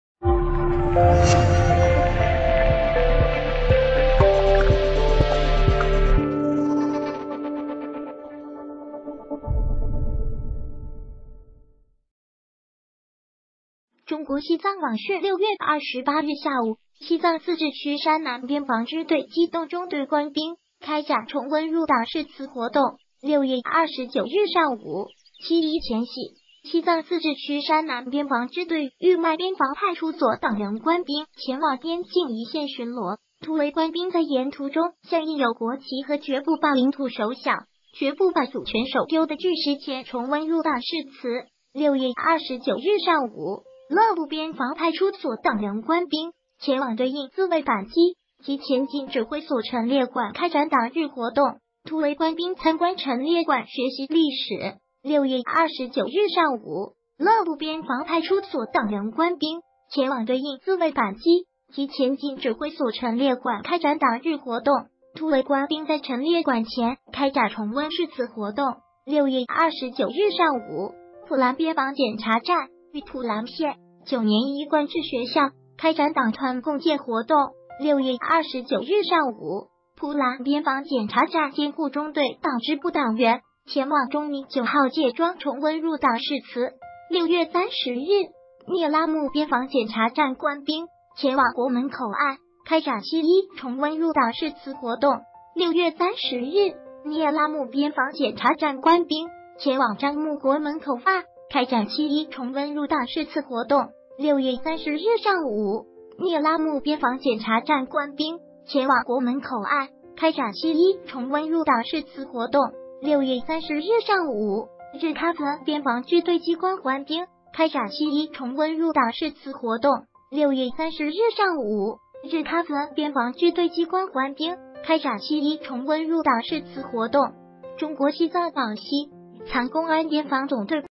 西藏边防总队庆“七一”重温入党誓词
谢谢！西藏边防总队庆“七一”重温入党誓词